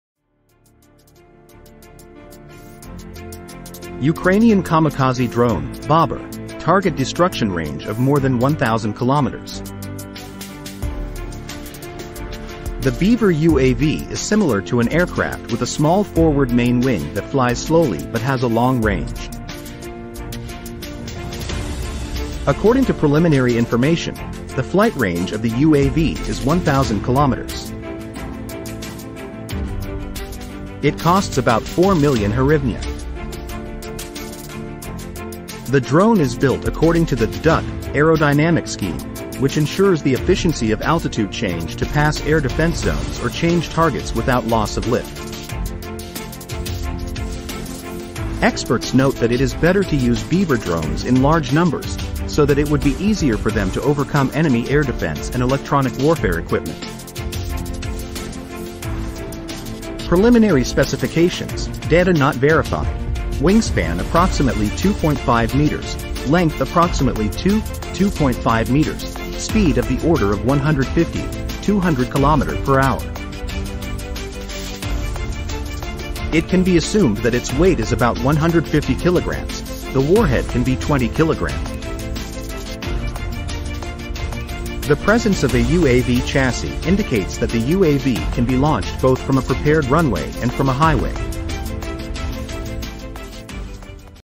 "Ukrainian Kamikaze Drone "Bober": Unleashing the Beaver UAV's Long-Range Fury!"
🚀 Introducing the "Bober" Ukrainian Kamikaze Drone - Unleashing Destructive Power Beyond 1000 km! 🚀

In this captivating video, we delve into the cutting-edge world of military drone technology as we explore the formidable "Bober" kamikaze drone. Designed to strike targets with unparalleled precision and reach, this lethal UAV boasts a target destruction range of over 1000 kilometers.

The "Bober" is part of the Beaver UAV family, characterized by its aircraft-like design with a small forward main wing that enables slow, steady flight while covering extensive distances. Armed with the potential to carry a 20 kg warhead, this drone proves to be a force to be reckoned with.

Built with the innovative "duck" aerodynamic scheme, the "Bober" excels in efficient altitude changes, allowing it to skillfully maneuver through air defense zones and switch targets without compromising lift.

As we unveil preliminary specifications (please note that data is yet to be verified), this drone boasts a wingspan of approximately 2.5 meters and a length of 2 to 2.5 meters. With a cruising speed ranging from 150 to 200 km/h, it exhibits the ideal blend of agility and endurance.

Costing around 4 million hryvnia, the "Bober" showcases the prowess of Ukrainian engineering and military ingenuity.

Join us on this exhilarating journey as we explore the strategic implications of deploying Beaver drones en masse, as experts propose. With the ability to challenge enemy air defense systems and electronic warfare equipment, the impact of these drones in large numbers is astounding.

The versatility of the "Bober" is further emphasized by its ability to launch from both prepared runways and highways, making it a potent asset for various mission scenarios.

Don't miss out on this exclusive insight into the Ukrainian kamikaze drone "Bober" and its revolutionary capabilities. 🎯🛩️🔥